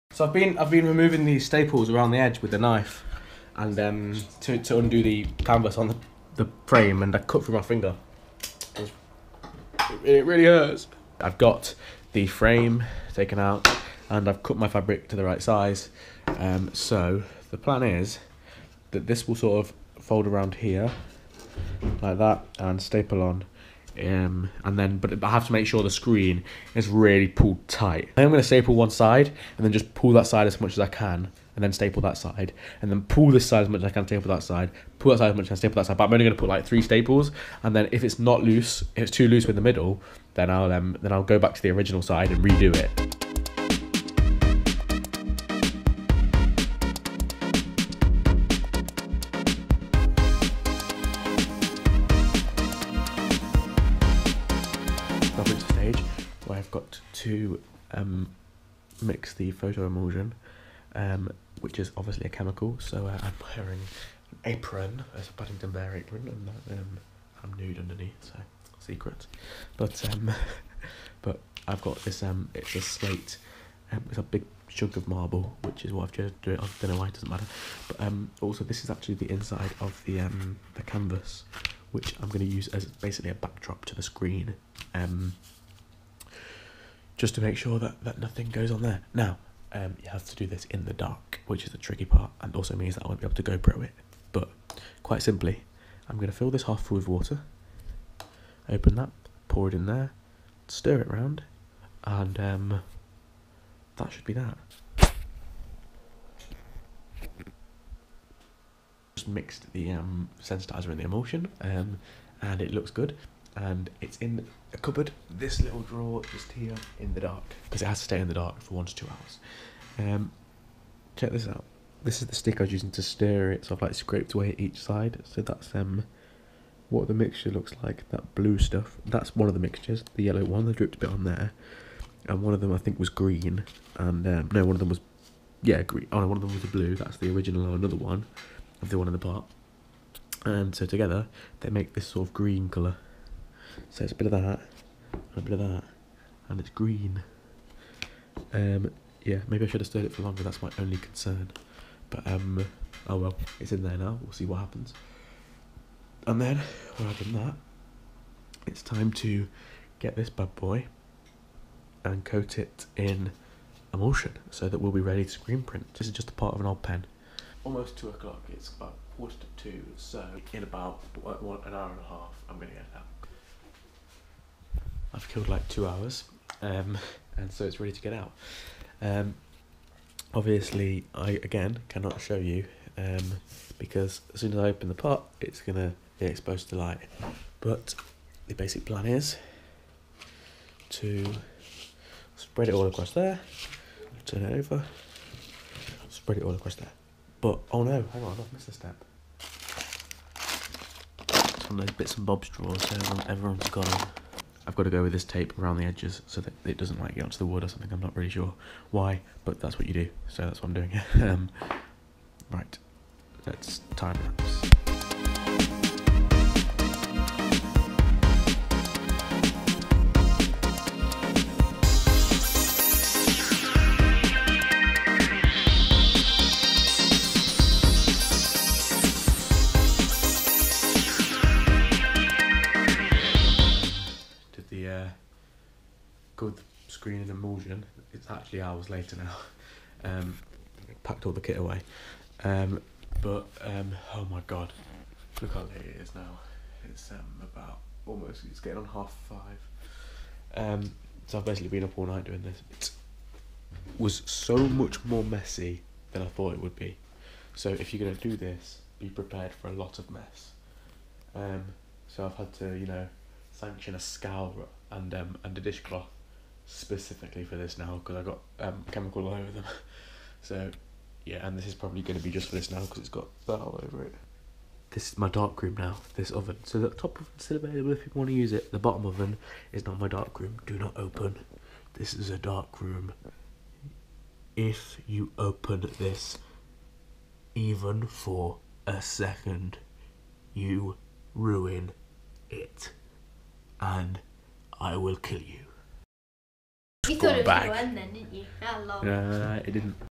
How to make Screen Printing Frames - MoPro 57
I work all night to create my own frames for screen printing.

Music from Official MoPro DJ Lawrence Cotter...